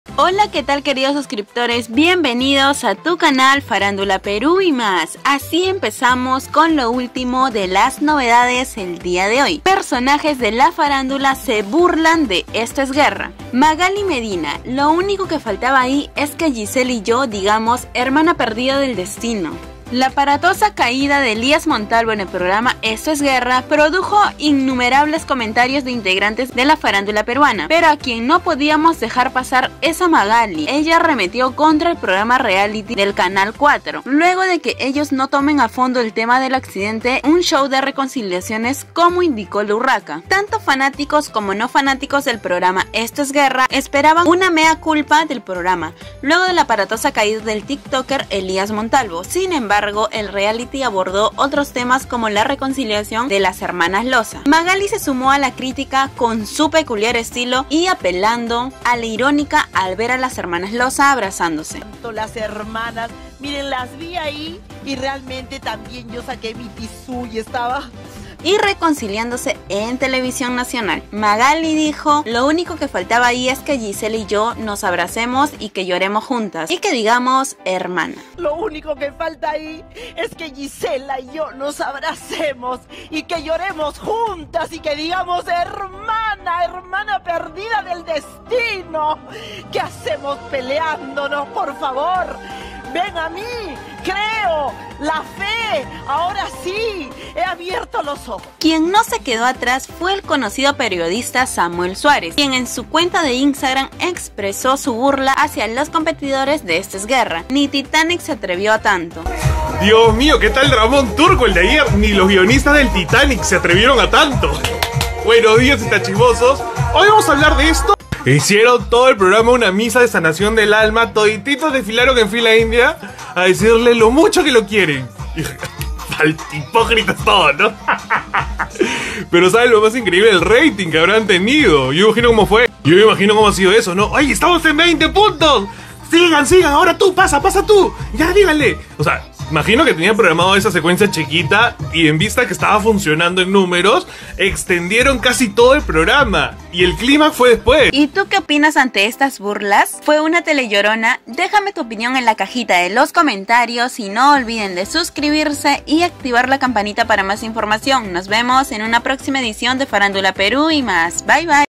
¡MAGALY MEDINA SE BURLA DE EEG POR "TELELLORONA" DE LAS HERMANAS LOZA! SE AMISTA CON GISELA?!
¡EXPLOTÓ y SE BURLA! MAGALY MEDINA SE BURLA DE LAS DRAMÁTICAS HERMANAS LOZA Y SU RECONCILIACION! SE BURLA DEL LLANTO DE LOS COMPETIDORES TRAS LA CAIDA DE ELIAS MONTALVO!! NADIE LES CREE! SAMU SUAREZ ESTALLÓ!